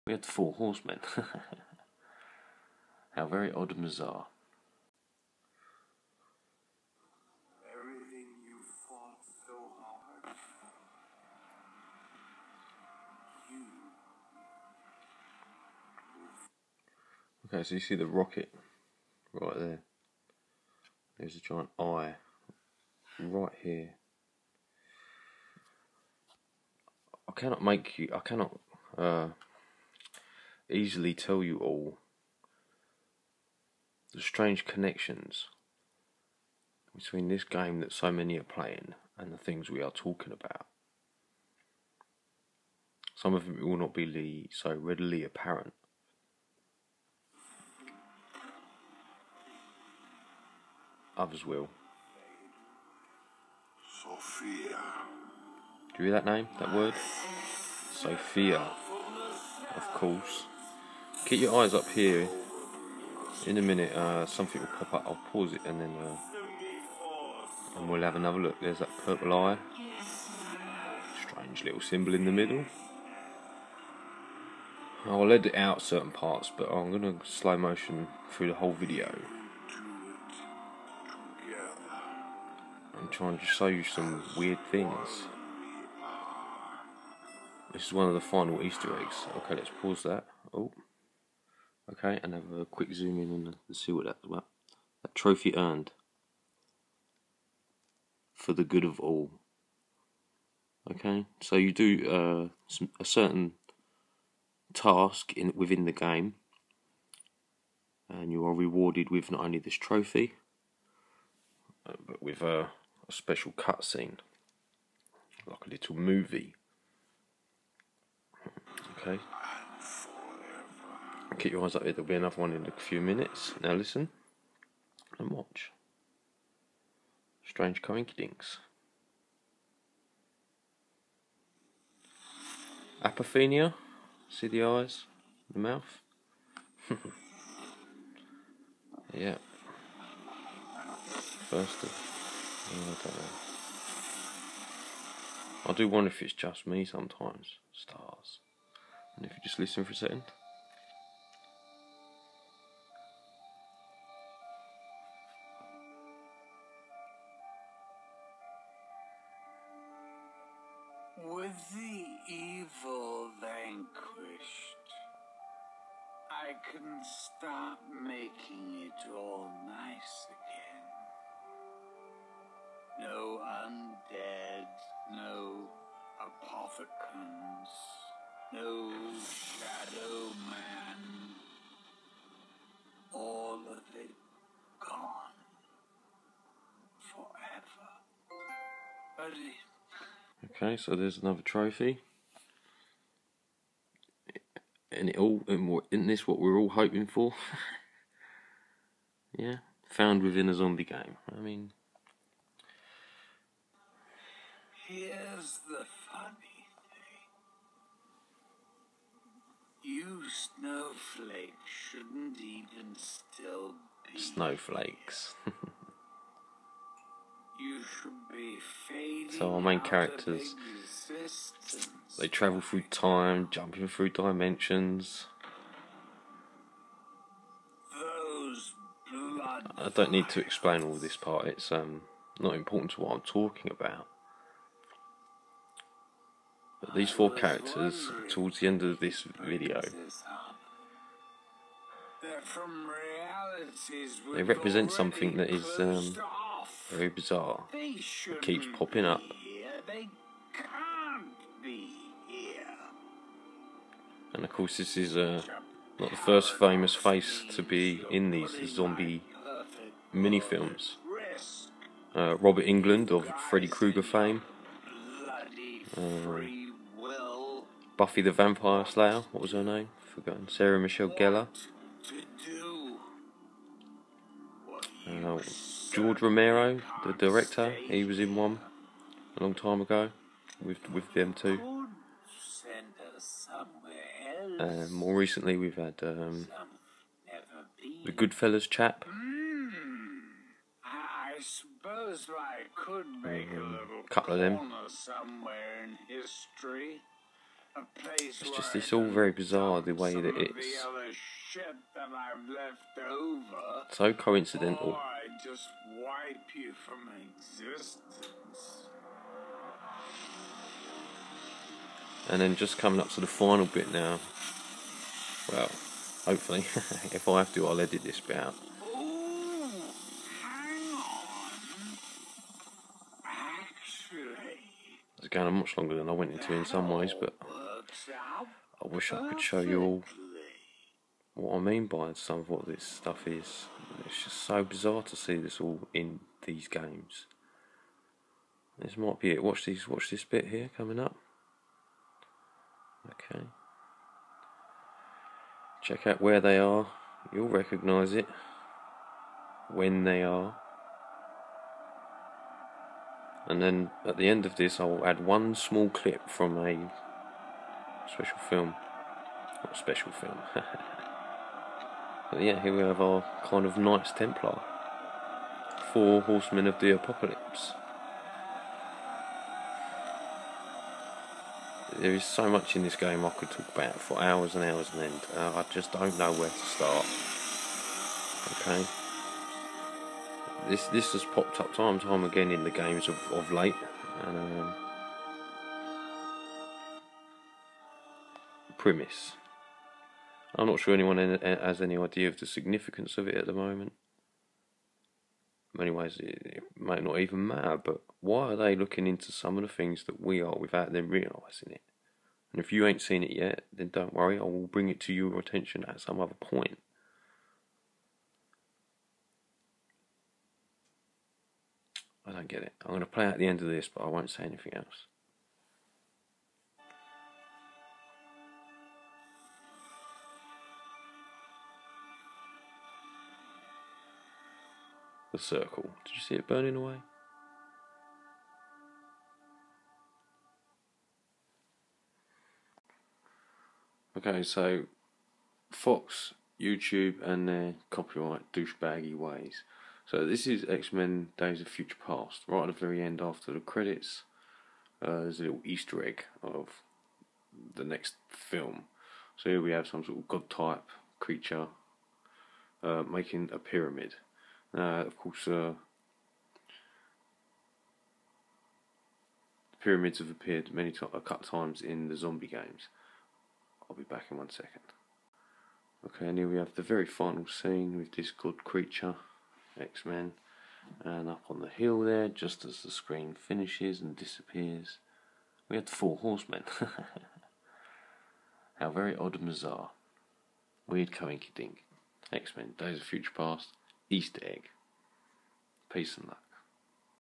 Flat Earth - Four The Good of All X-Men & A Better Tomorrow ... ???
Flat Earth Connections in CoD Zombies & X-Men Days of Future Past ... ???